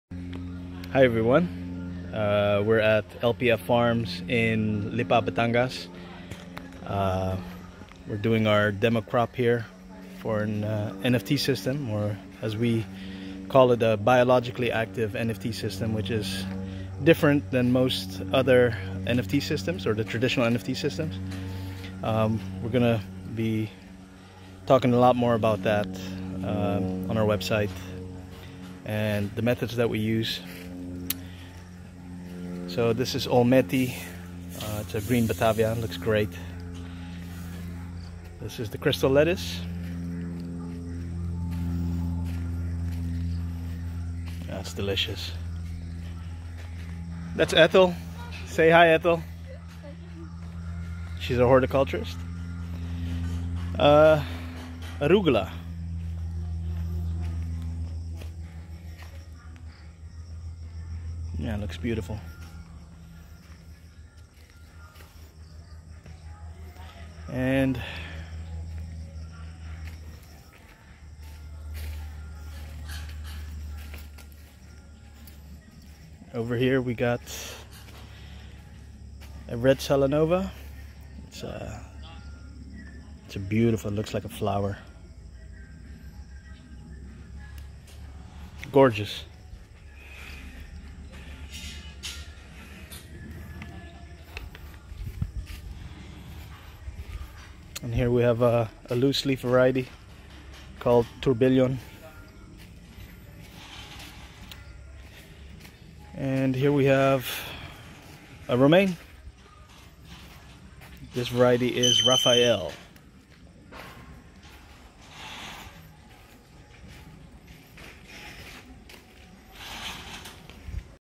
Growing hydroponics lettuce and arugula at La Petite Ferme greenhouse in Lipa, Batangas
We have spent lots of time and effort into transcending our grows from conventional hydroponics chemistry to involve the micro biology a lot more to improve the quality of produce. It's no longer a race for us to chase after big monstrous heads of lettuce rather, we're looking to create great-tasting lettuce with a proactive approach to fertilizer efficiency and environmental care by getting more involvement of the microbes in a hydroponic setting. We're happy with our evolution into  the regenerative sphere of agriculture and the results speak for themselves.

Feed schedule 12x per day for 10 mins with a nutrient solution of 150ppm N in 6.0 pH and 1.5 EC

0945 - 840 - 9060